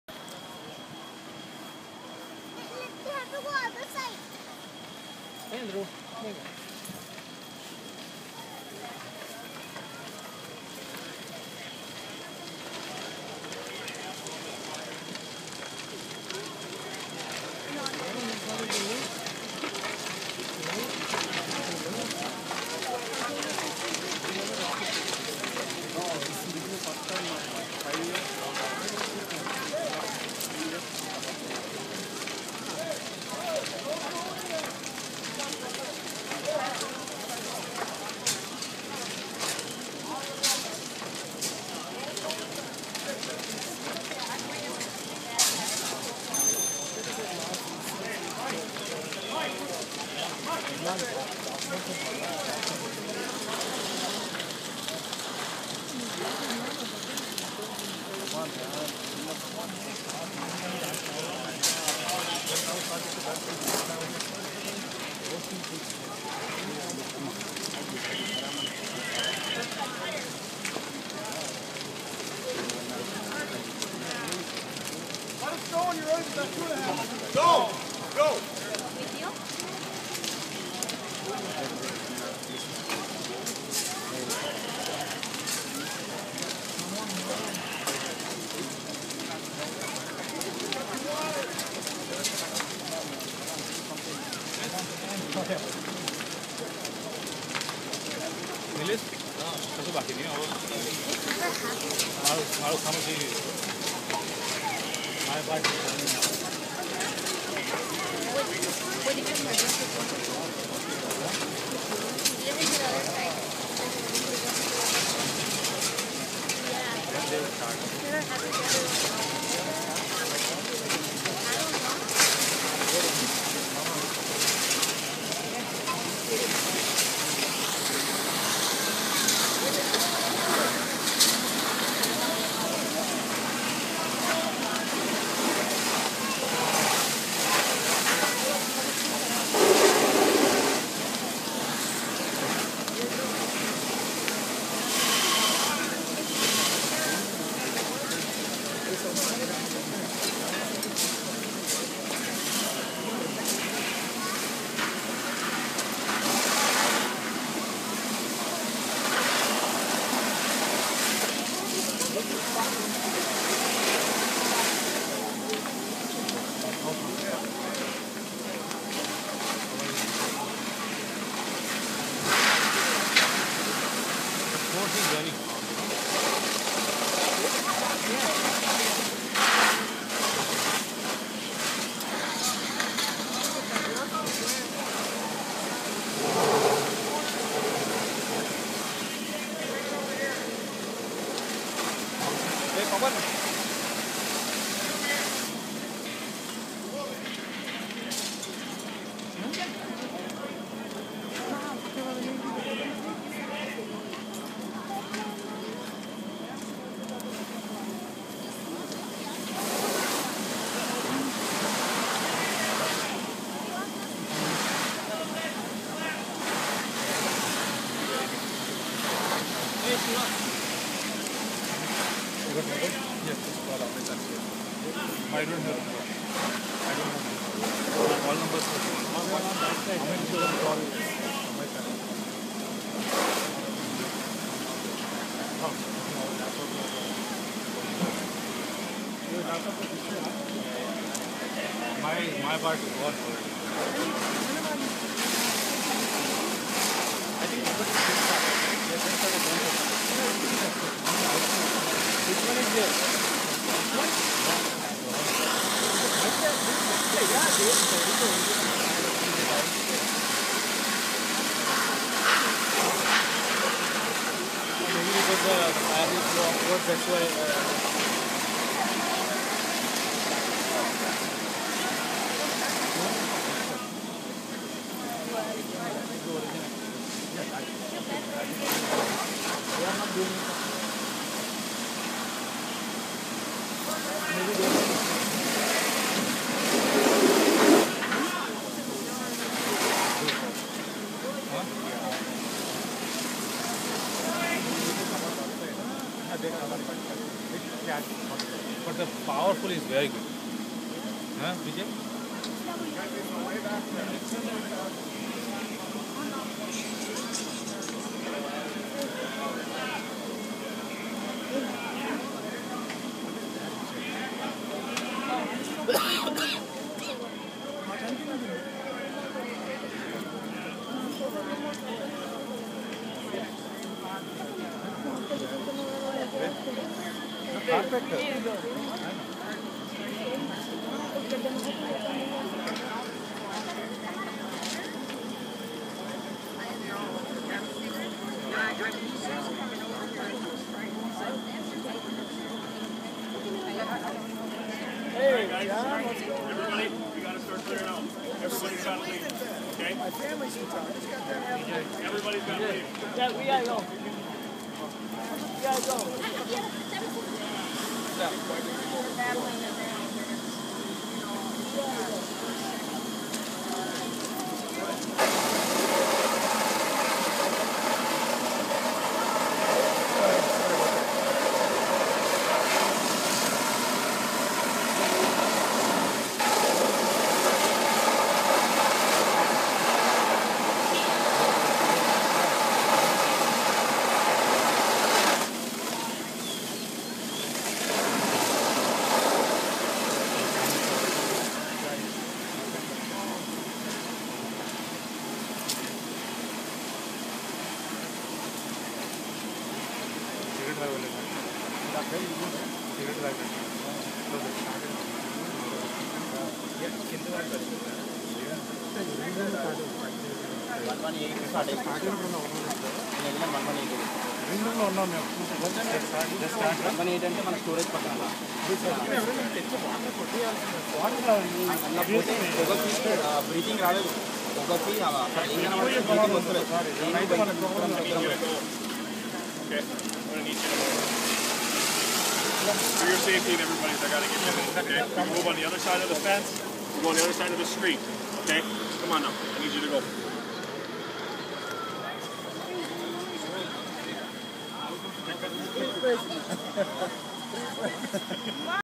4141 Parklawn Avenue Fire, Edina, Minnesota
This fire started shortly after 11 p.m. on July 10, 2012 at the Parklawn Estates building (4141 Parklawn Avenue, Edina, MN).

The fire started in a first floor rental unit when no one was home and quickly spread to the second and third floor of the apartment complex. Everyone made it out safely.

Portions of this video were aired on both WCCO Channel 4 and KARE 11 news the morning of July 11th, 2012. Here are those stories: